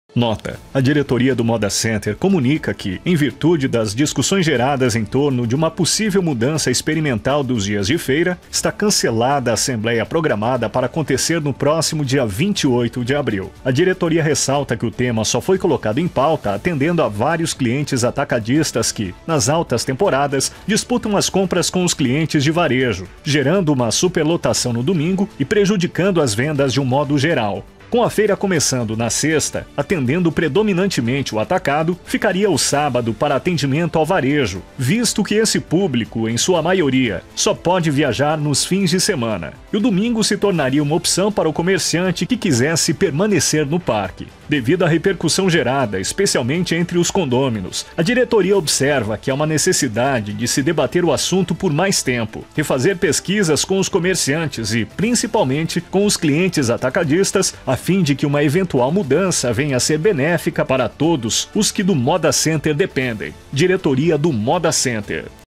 Comunicado do cancelamento da assembleia do dia 28/04/2022
A diretoria do Moda Center comunica que, em virtude das discussões geradas em torno de uma possível mudança experimental dos dias de feira, está cancelada a assembleia programada para acontecer no próximo dia 28 de abril.

A diretoria ressalta que o tema só foi colocado em pauta atendendo a vários clientes atacadistas que, nas altas temporadas, disputam as compras com os clientes de varejo, gerando uma superlotação no domingo e prejudicando as vendas de um modo geral.

Com a feira começando na sexta, atendendo predominantemente o atacado, ficaria o sábado para atendimento ao varejo (visto que esse público, em sua maioria, só pode viajar nos fins de semana) e o domingo se tornaria uma opção para o comerciante que quisesse permanecer o parque.

Devido à repercussão gerada, especialmente entre os condôminos, a diretoria observa que há uma necessidade de se debater o assunto por mais tempo, refazer pesquisas com os comerciantes e, principalmente, com os clientes atacadistas, a fim de que uma eventual mudança venha a ser benéfica para todos os que do Moda Center dependem.

Diretoria do Moda Center - Santa Cruz do Capibaribe, 18 de abril de 2022.